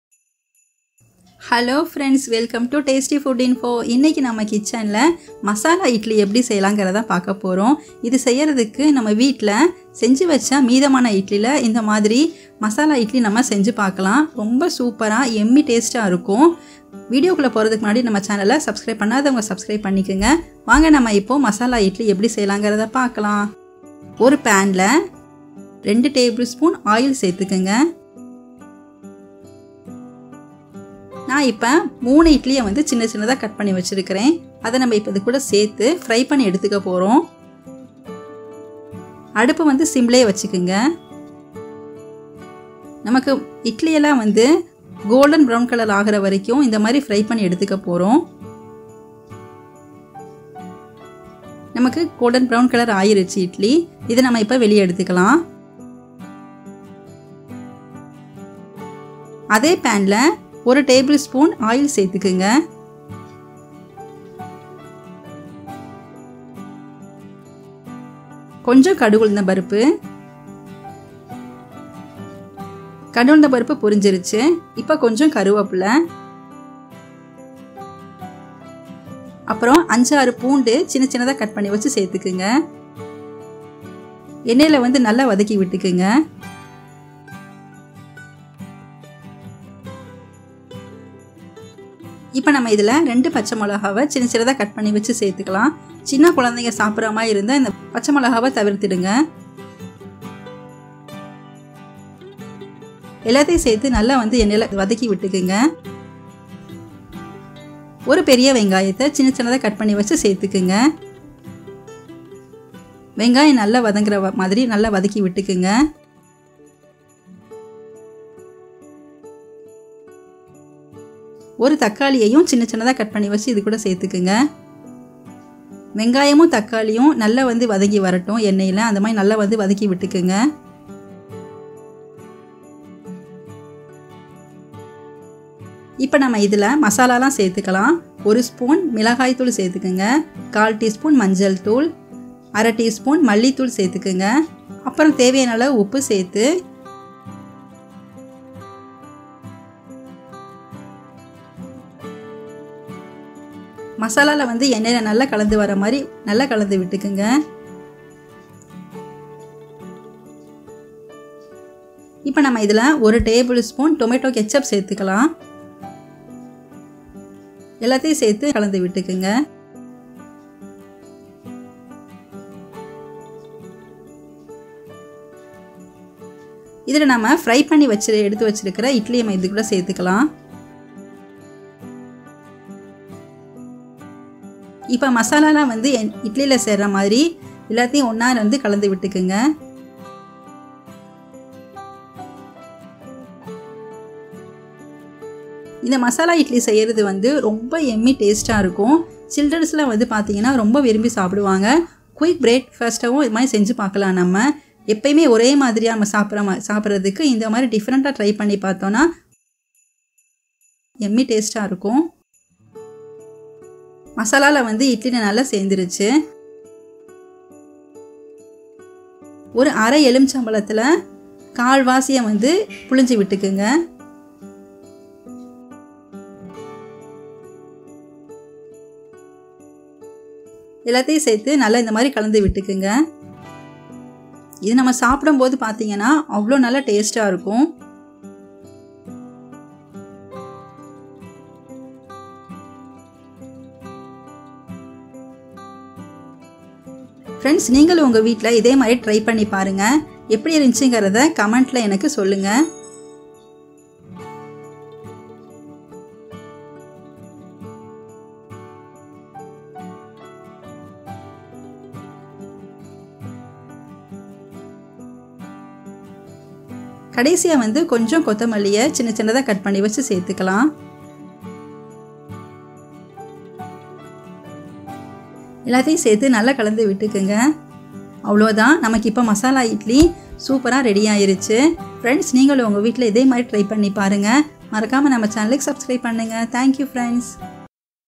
masala fried idli in tamil | Breakfast Recipes |tasty food info
Ingredients

Idli – 3
garlic – 5
Finely chopped onion – 1
Tomato – 1
Salt
Tomato ketchup – 1 tbl spoon
Coriander leaves
curry leaves
green chilli
mustard seeds
lemon- 1/2